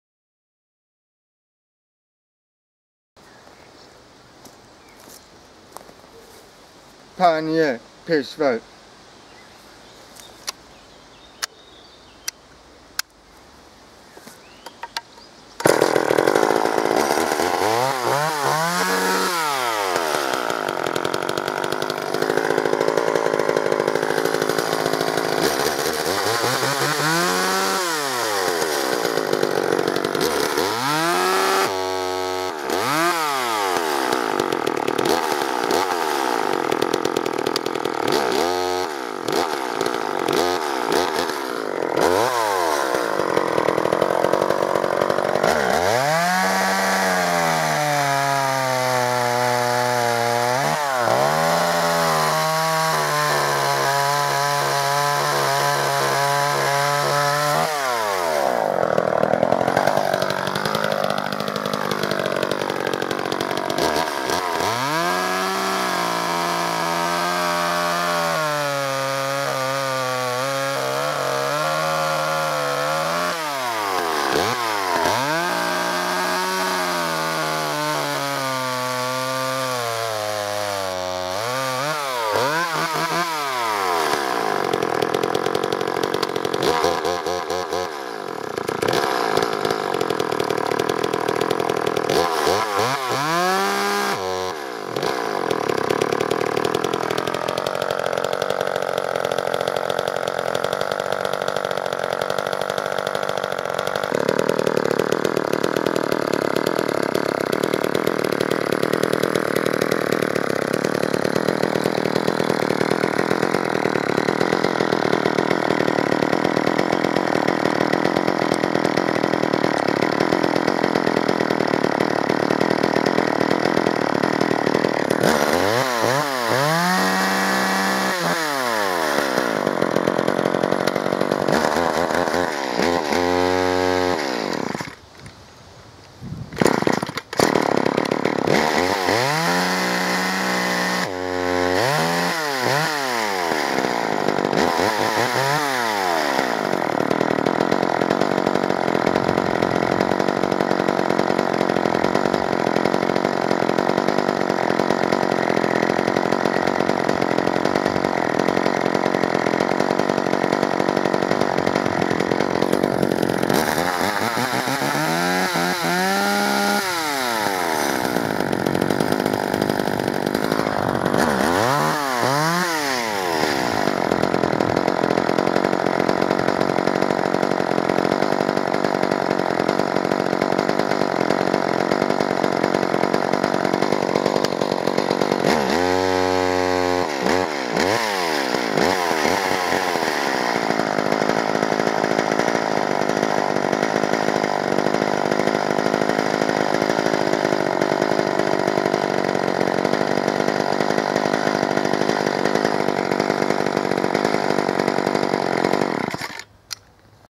Pioneer P12 Oldtimersäge
Die Pioneer P12 wurde von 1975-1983 gebaut.
TECHNISCHE DATEN:
Hubr.: 34 ccm
PS: 2
U/min.: 10.000
Gew.: 3,6 kg
Kraftstoff: 1:25